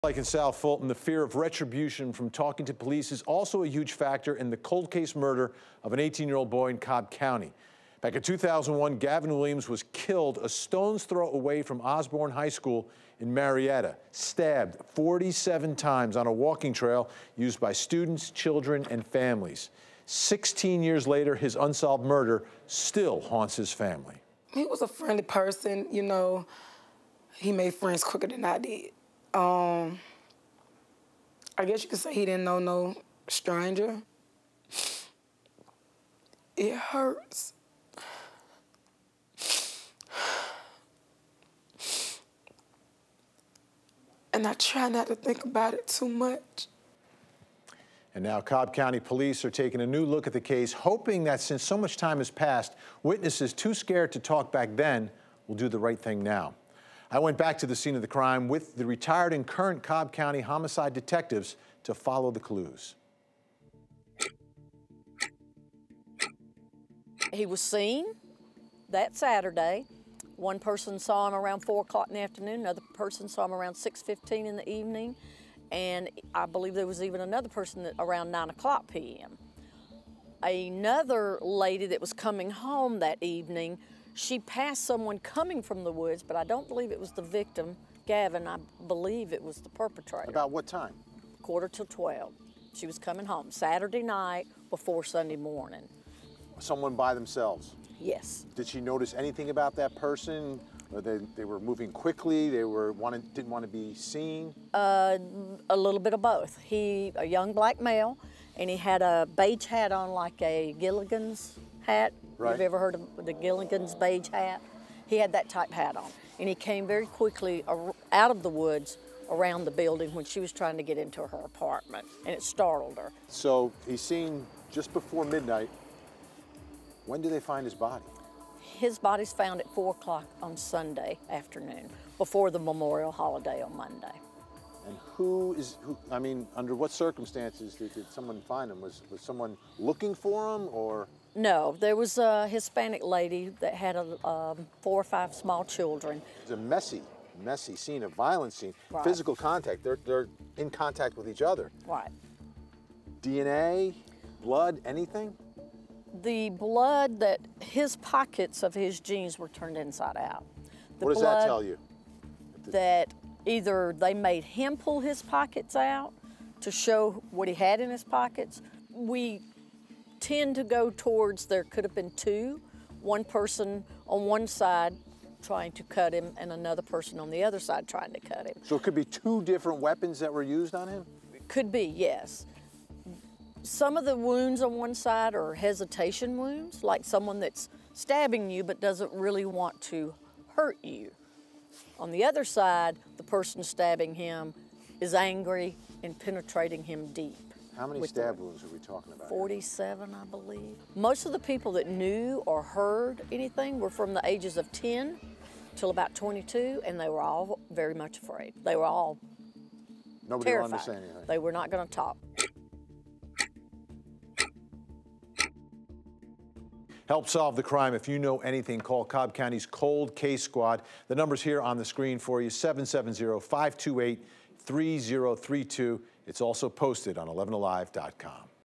TheLateFeed: Police take second look at 2001 cold case of teen's stabbing
11Alive went back to the scene of the gruesome crime.